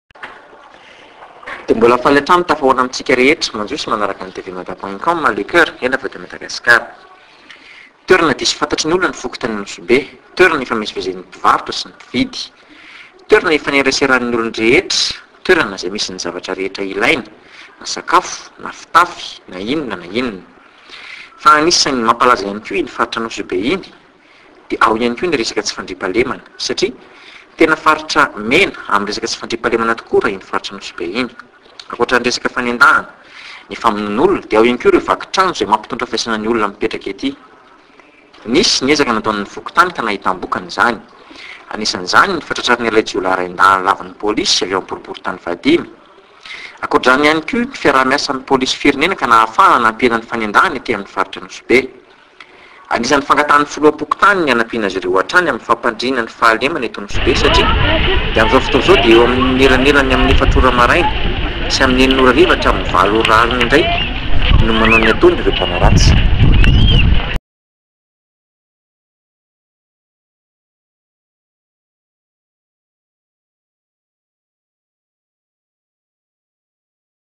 DÉVELOPPEMENT : LE FOKONTANY MANDRAGOBATO I - ANOSIBE
Mandragobato I se trouve dans la commune urbaine d’Anosibe dans la capitale de Madagascar, il fait parti des bas quartiers où s’entassent tous les laisser pour compte issus de l’exode rurale. Et pourtant, toute cette zone a été réhabilitée en 2004 avec la mise au norme de la route nationale 1 qui traverse cette localité.

La population qui habite cet endroit est ainsi victime quotidiennement de tous les maux du sous-développement tels qu’une insécurité totale, un trafic de drogue envahissant, une pauvreté accrue, un accès à l’eau difficile, une assistance aux soins caduque, un service éducatif mal réparti, etc… sans parler de la corruption chronique.

C’est un cercle vicieux pour s’en sortir de ce chaos lorsqu’on se trouve dans un environnement aussi destructeur et pourtant beaucoup de gens ne se laissent pas abattre et quelques organismes humanitaires apportent leurs aides pour améliorer la vie quotidienne des habitants et leurs donnent de l’espoir d’un avenir meilleur.

M.R.

Toerana diso fantatry ny olona ny fokontany Mandragobato 1 eny Anosibe iny dia toerana hifamezivezen’ny mpivarotra sy ny mpividy. Toerana hifaneraseran’ny olon-drehetra, toerana izay misy ny zavatra rehetra ilaina na sakafo na fitafy na inona na inona. Fa anisan’ny mampalaza ihany koa iny faritra eny Anosibe iny ihany koa ny resaka tsy fandriampahalemana. Satria tena faritra mena amin’ny resaka tsy fandriampahalemana tokoa raha iny faritra Anosibe iny.

Ankoatra ny resaka fanendahana, ny famonon’olona dia ao ihany koa ireo vaky trano izay mampitondra faisana ny olona mipetraka ety. Nisy ny ezaka nataon’ny fokontany ka nahitam-bokany izany. Anisan’izany ny fahatratrarana ilay jiolahy raindahiny lavon’ny polisy avy ao amin’ny boriboritany faha dimy. Ankoatra izany ireo fiaraha miasa amin’ny polisim-pirenena ka nahafahana nampihena ny fanendahana ety amin’ny faritra Anosibe.

Anisan’ny fangatahan’ny filoham-pokontany ny hanampiana azy ireo hatrany amin’ny fampandriana ny fahalemana eto Anosibe satria dia amin’izao fotoana izao dia eo amin’ny amin’ny 4ora maraina sy amin’ny 6ora hariva hatramin’ny 8 ora alina indray no manao ny hataony ireo mpanao ratsy.

Andoniaina Valy

Mandragobato I is located in the urban commune of Anosibe in the capital of Madagascar, it is part of the low districts where pile up all the leaves from the exodus rural. Yet the whole area was rehabilitated in 2004 with the upgrading of the national road 1 through this locality.

The population living in this area is thus a daily victim of all the evils of underdevelopment, such as total insecurity, invasive drug trafficking, increased poverty, difficult access to water, decadent care, Poorly distributed educational service, etc., not to mention chronic corruption.

It is a vicious circle to get out of this chaos when we are in such a destructive environment and yet many people are not let down and a few humanitarian organizations are helping to improve the daily lives of the inhabitants and their families. Give hope for a better future.